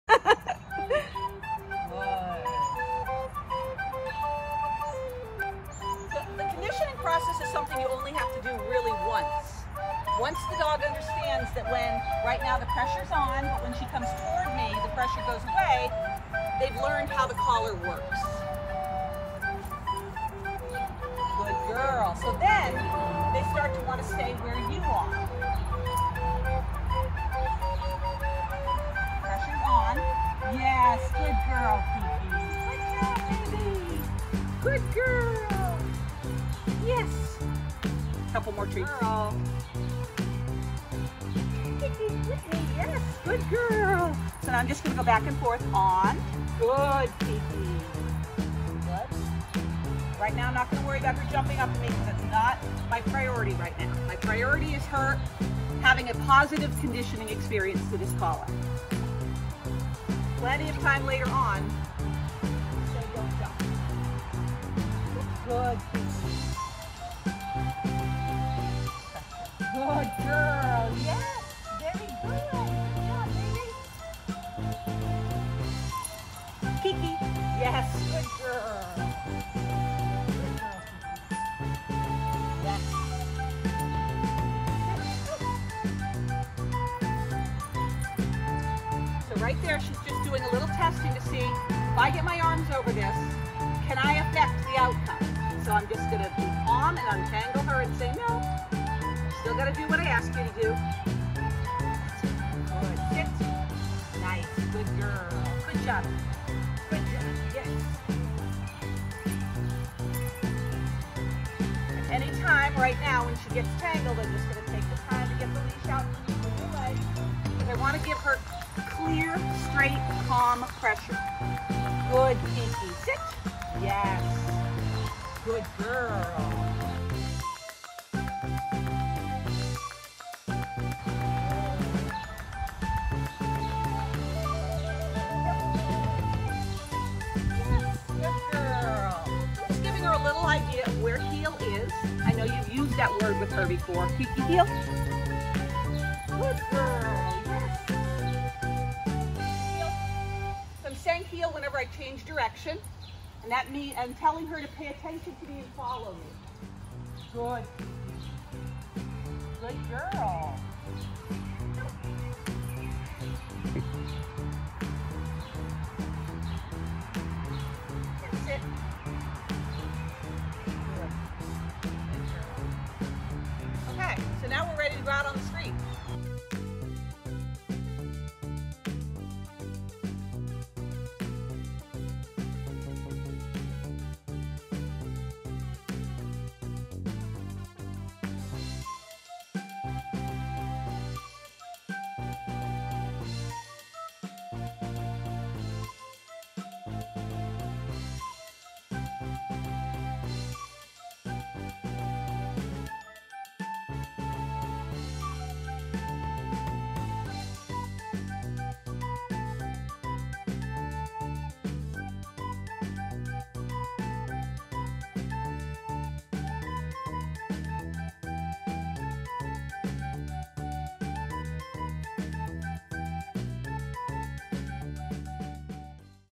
Conditioning the Prong Training Collar - Ep. 207
The "big scary" Prong Collar conditioning process revealed... Prong collars: misused or misjudged?